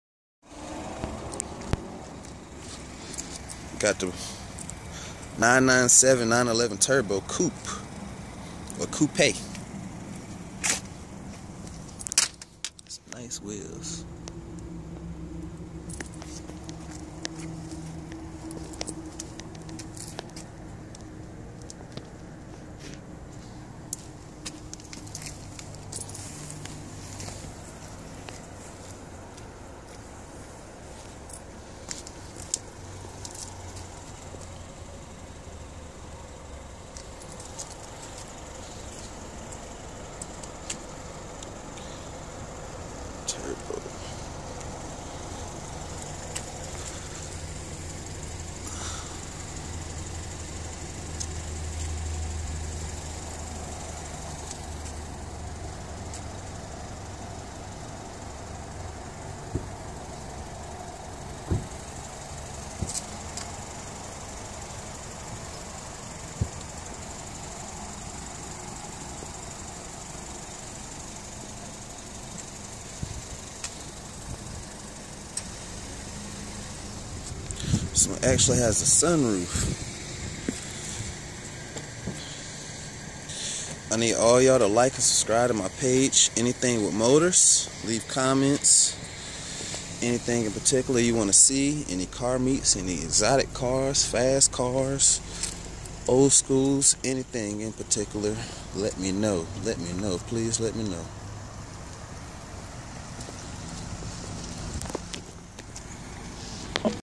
Porsche 911 turbo the best everyday supercar
Come check out this nice Porsche 911 turbo pdk 500hp 4wd with back seats at Porsche automall of Cary tell them Gio sent ya.        Facebook.com/AnythingWithMotors.     Y9utube.com/Anythingwithmotors